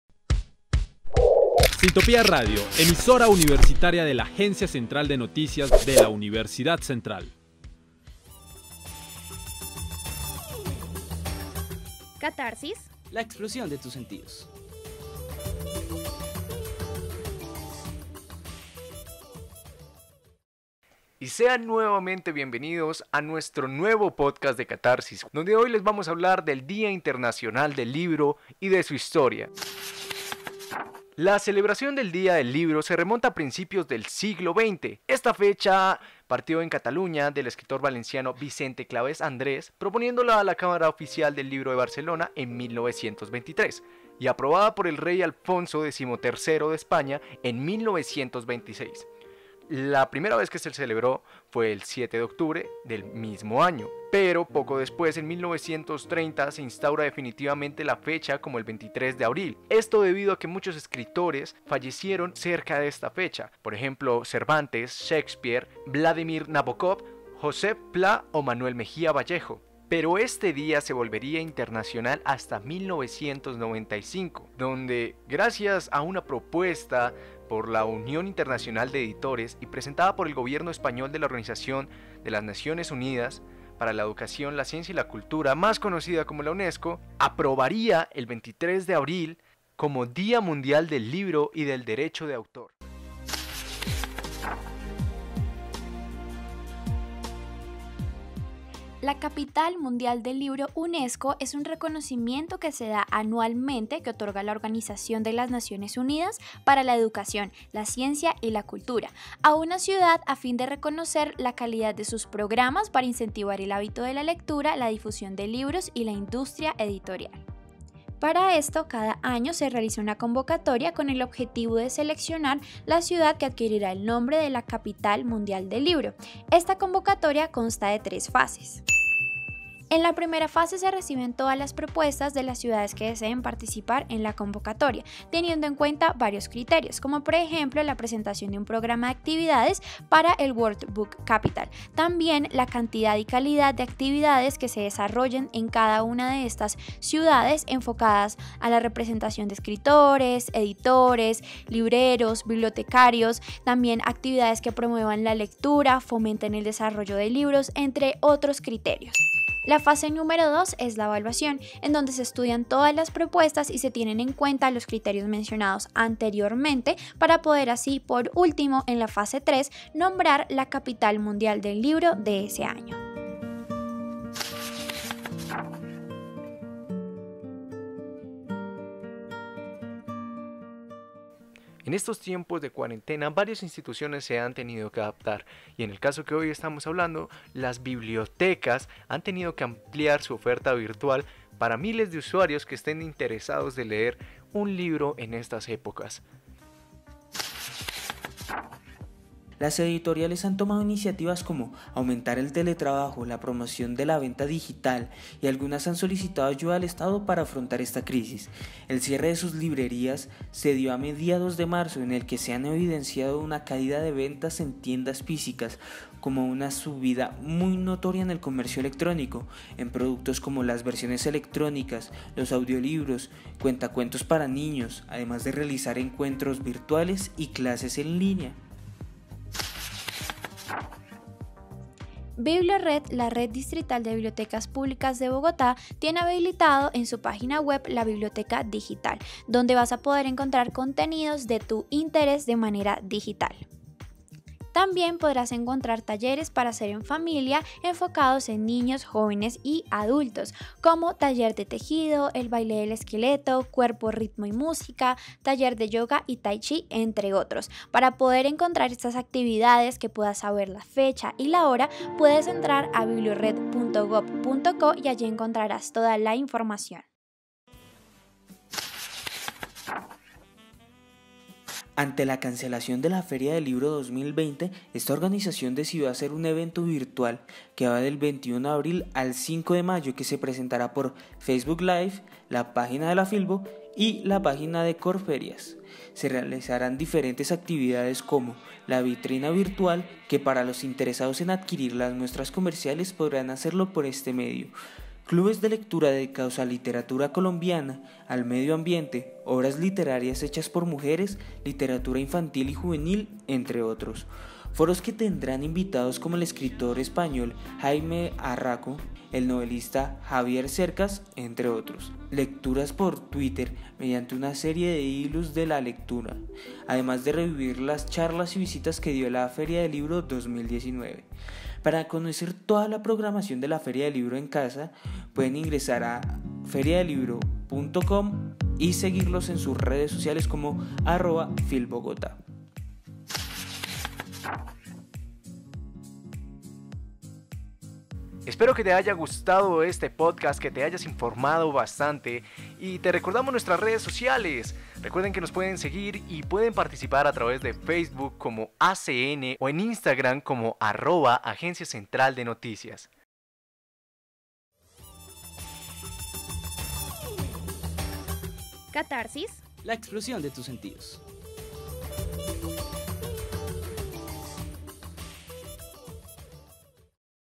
Sintopía: Día internacional del libro
El día internacional del libro se viene celebrando desde el 23 de abril 1930, aunque la UNESCO aprobó este día solo hasta 1995; desde entonces se viene escogiendo cada año una ciudad diferente para que represente como la capital mundial del libro, reconociendo sus programas para promoción de la lectura.

En estos tiempos de cuarentena diferentes instituciones como las editoriales y las bibliotecas han tomado diferentes estrategias para seguir brindado sus servicios, por ejemplo han ampliado su oferta virtual o dar clases en línea de diferentes actividades